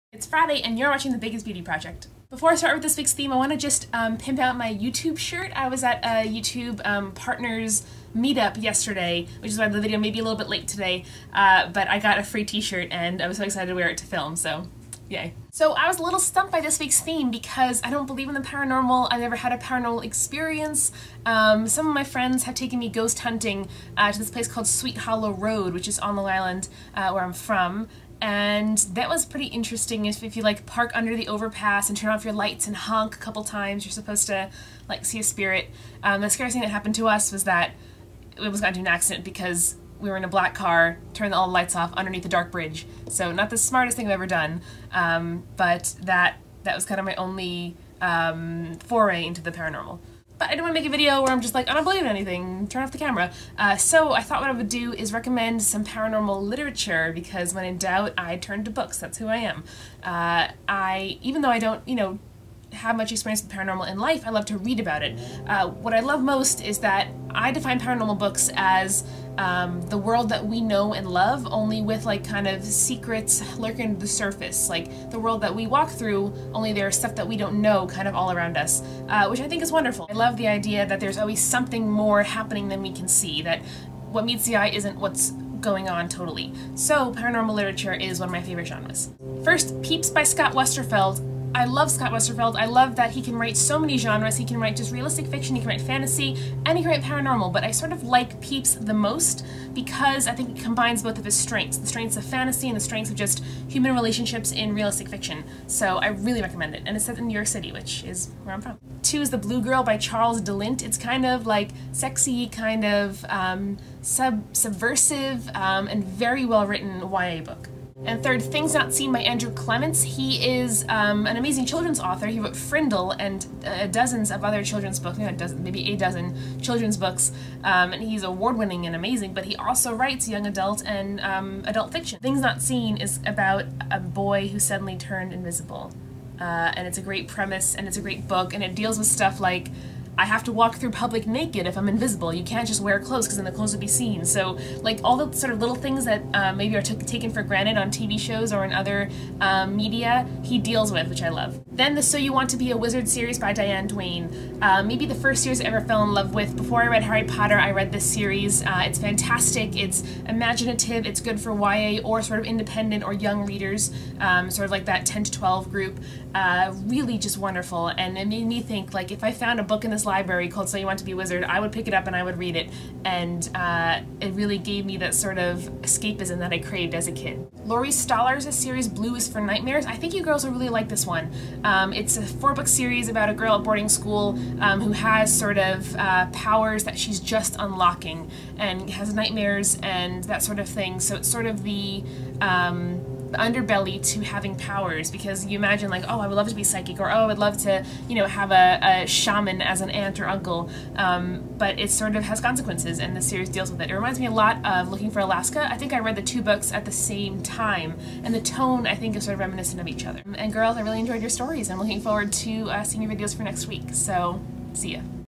Friday Loves Paranormal Lit
Amanda recommends some paranormal YA and children's lit, 'cause that's how she rolls.